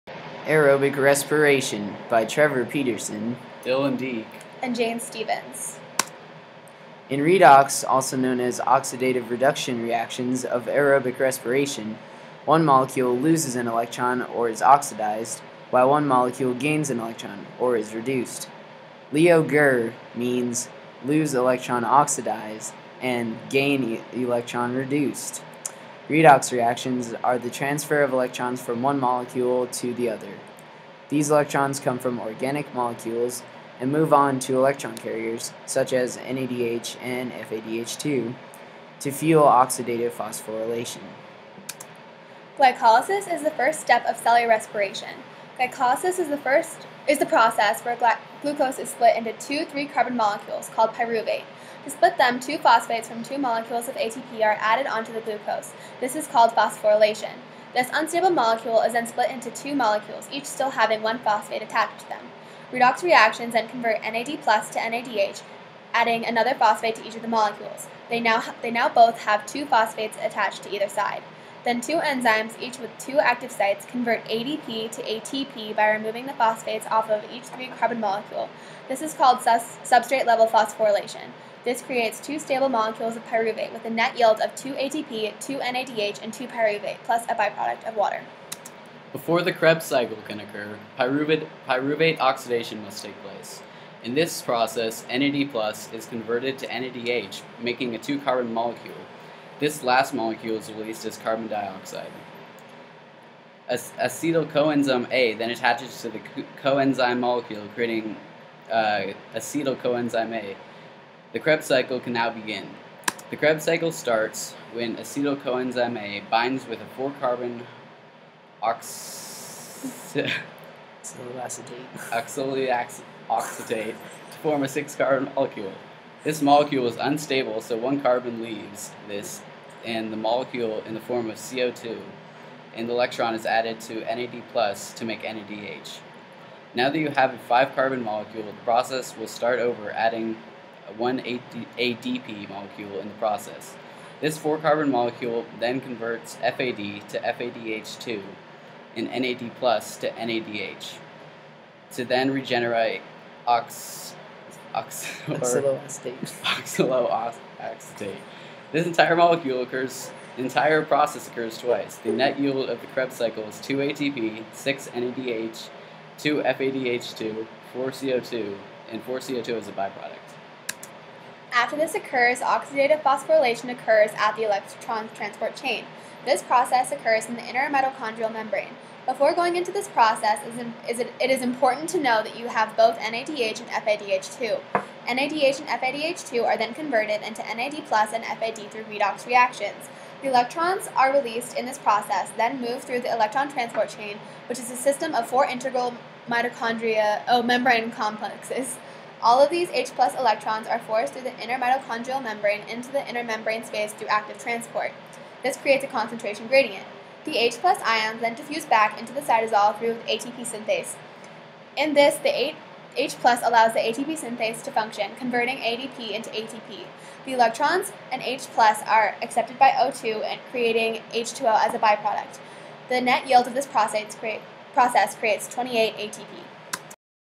Aerobic Respiration
Aerobic respiration, by Trevor Peterson, Dylan Dieck, and Jane Stephens.

In the Redox (oxidation reduction) Reactions of aerobic respiration, one molecule loses an electron, or is oxidized; while one molecule gains an electron, or is reduced.
LEO GER means Lose Electron Oxidized and Gains Electron Reduced.
Redox reactions are the transfer of electrons from one molecule to the other. These electrons come from organic molecules and move on to electron carriers (NADH and FADH2) to fuel oxidative phosphorylation.

Glycolysis is the first step of cellular respiration.
Glycolysis is the process where glucose is split into two 3 carbon molecules, called pyruvate. To split them, two phosphates from two molecules of ATP are added on to the glucose. This is called phosphorylation.This unstable molecule is then split into two molecules, each still having one phosphate attached to them. Redox reactions then convert NAD+ to NADH, adding another phosphate to each of the molecules. (They now both have two phosphates attached to either side). Then two enzymes, each with two active sites, convert ADP to ATP by removing the phosphates off of each 3-carbon molecule. This is called substrate level phosphorylation. This creates two stable molecules of pyruvate, with a net yield of 2 ATP, 2 NADH, and 2 pyruvate, plus a byproduct of water.

Before the Krebs cycle can occur, Pyruvate oxidation must take place. In this process NAD+ is converted to NADH making a two carbon molecule this last molecule is released as carbon dioxide. Acetyl co-enzyme A then attaches to the two carbon molecule creating creating acetyl co-enzyme A. the Krebs cycle can now began. The Krebs cycle starts when acetyl co-enzyme A binds with a four carbon oxaloacetate to form a 6 carbon molecule. This molecule is unstable so 1 carbon leaves this the molecule in the form of Co2 and the electron is then added to NAD+ to make NADH. Now that you have a 5 carbon molecule the process will start over adding one ADP molecule in the process.this four carbon molecule then converts FAD to FADH2 and NAD+ to NADH. to then regenerate Oxaloacetate. This entire molecule occurs twice. The net yield of the Krebs cycle is 2 ATP 6 NADH 2 FADH2 4 CO2 as a byproduct.

After this occurs, Oxidative phosphorylation occurs at the electron transport chain. This process occurs in the inner-mitochondrial membrane. Before going into this process it is important to know that you have both NADH and FADH2. NADH and FADH2 are then converted into NAD+ and FAD+ through redox reactions. The electrons that are released in this process then move through the ETC which is a system of four integral membrane complexes. All of these H+ electrons are forced through the inner-mitochondria membrane into the inner-membrane space through active transport. This creates a concentration gradient. The H+ ions then diffuse back into the cytosol through ATP synthase. In doing this the, H+ allows the ATP synthase to function, converting ADP into ATP. The electrons and H+ are accepted by O2 creating H2O as a byproduct. The net yield of this process creates 28 ATP.